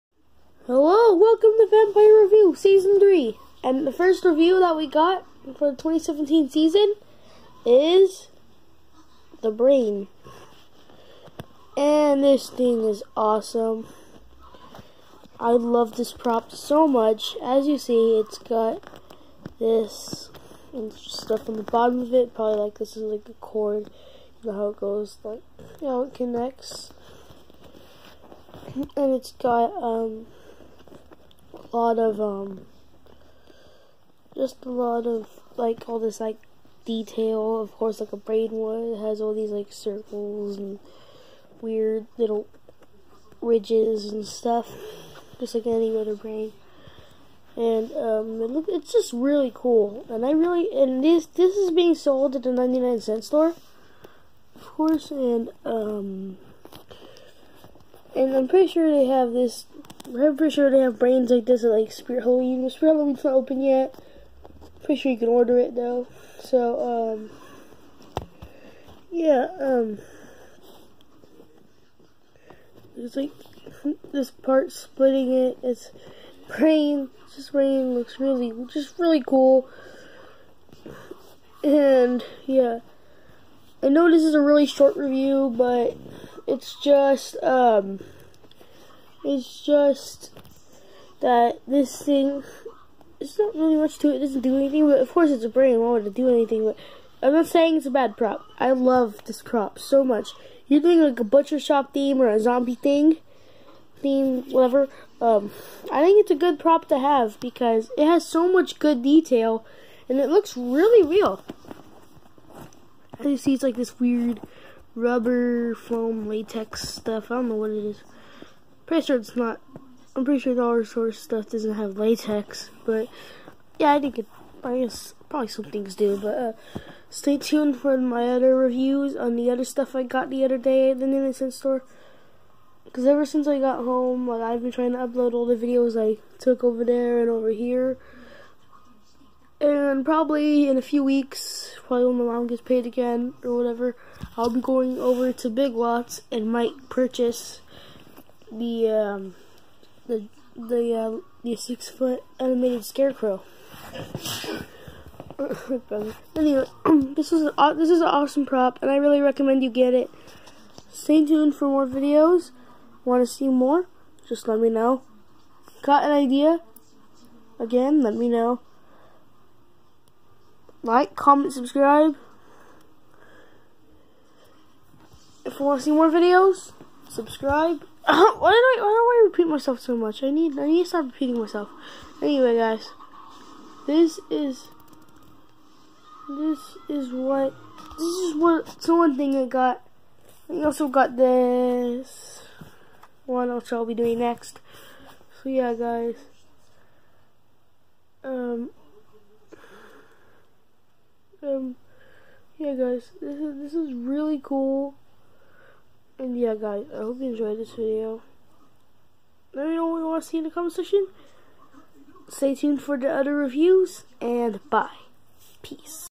Vampire reviews season 3: brain
Vampire reviews are back for a another awesome season! So watch out because some of these terrifying props could pounce! Just like this brain that you can purchase at the 99 cent only store!